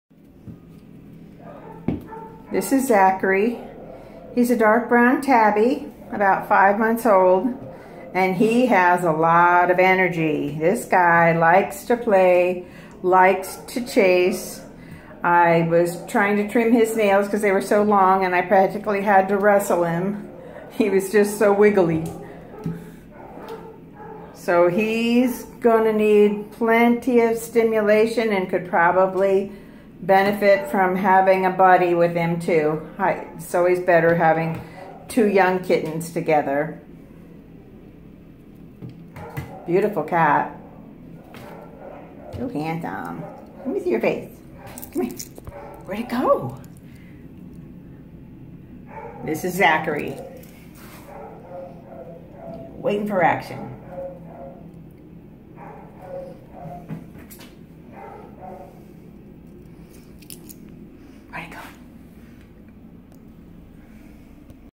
Zachery A746477 (ADOPTED 8/31/18)
Outgoing and rambunctious adolescent kitten who will need an adopter who will help expunge his energy. He arrived to the shelter with Marco (A746478), an equally energetic adolescent kitten.